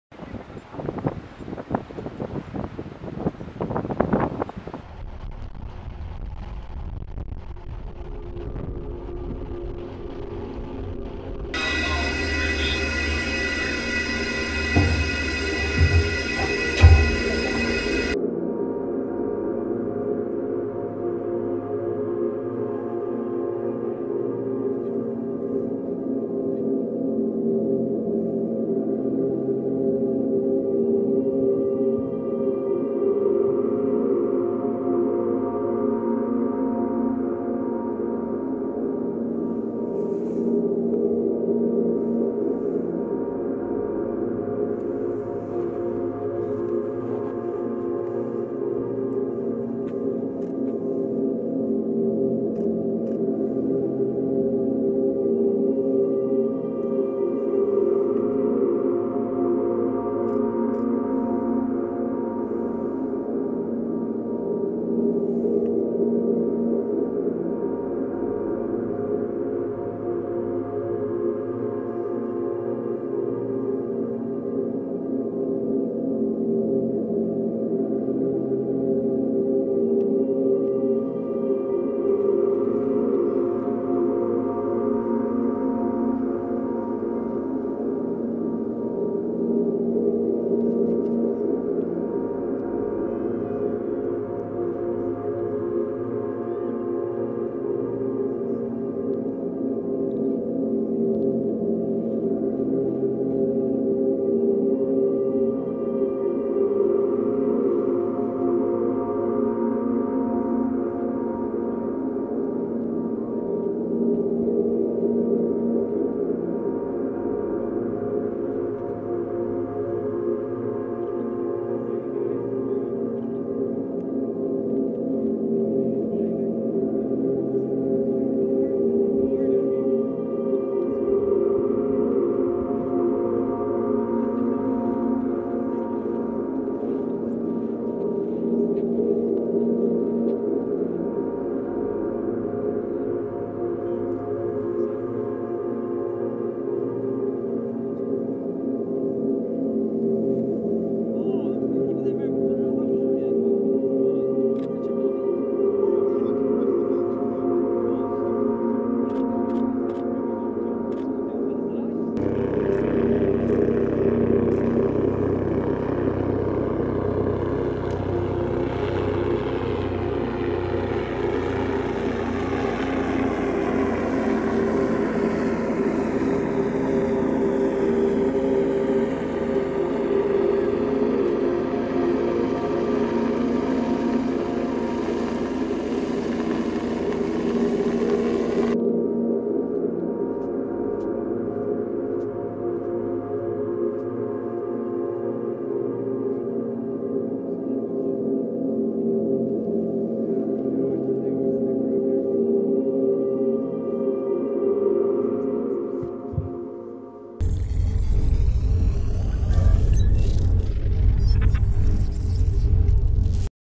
Navy submarine exercises appear in the Arctic Ocean
Military US - A British-flagged submarine blocks a layer of ice 60 centimeters thick and rises to the surface of the Arctic Ocean.

joint training with two US submarines, USS Connecticut and USS Hartford, which aims to test the ability of the submarine and its crew in combat under the ice.

In the exercise, the British Navy also tested submarine devices, especially sonar, targeting targets and training in tracking and fighting other submarines.

Joint exercises with the US Navy submarine became a rarity for Britain, although the US did similar exercises every two years.